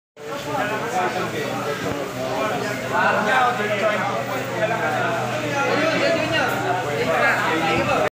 Olsen Racela and Rajko Toroman catch up with each other
Olsen Racela and Rajko Toroman once worked together at Petro Blaze Boosters